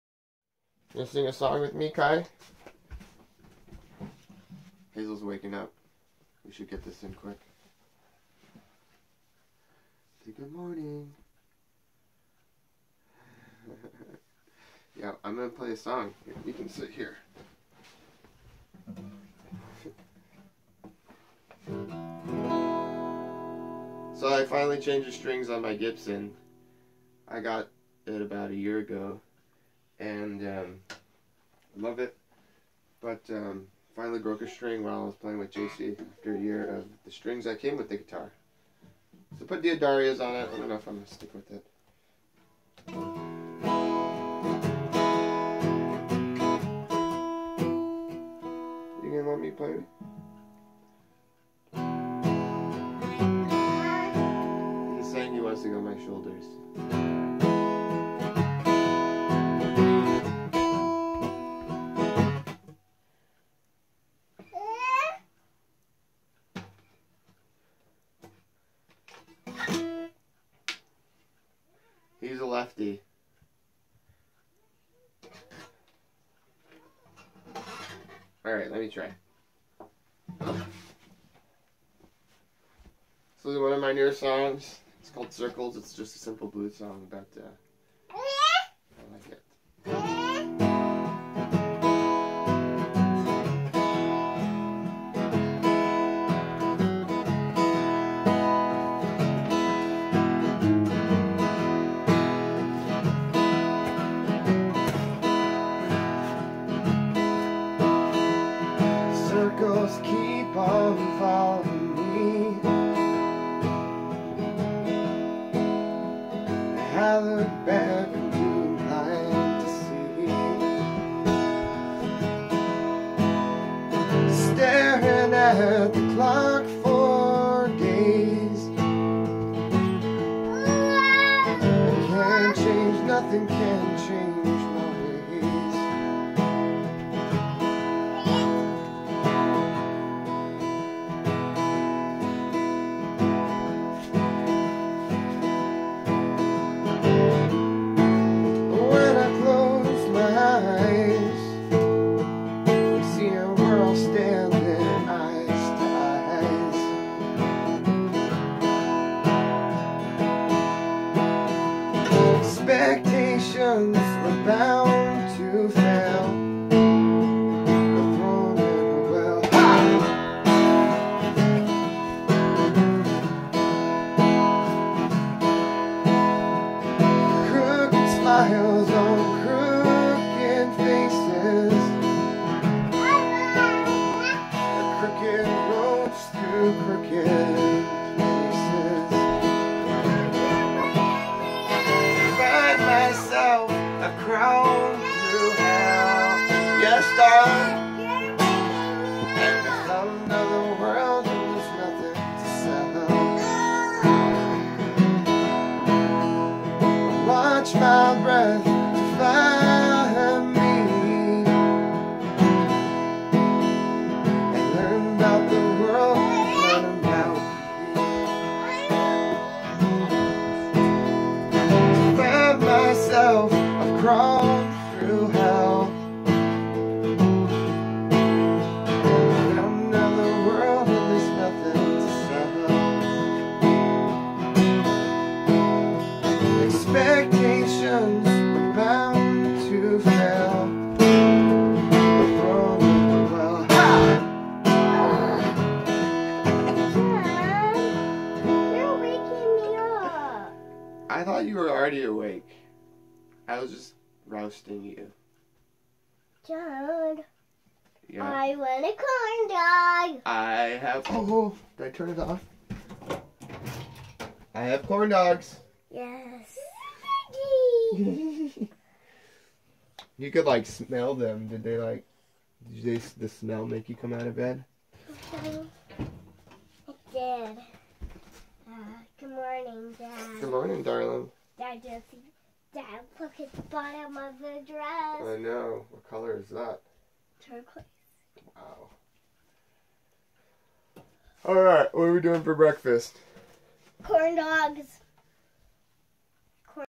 Singing a song and rousting the kids with corn dog
Loving family life.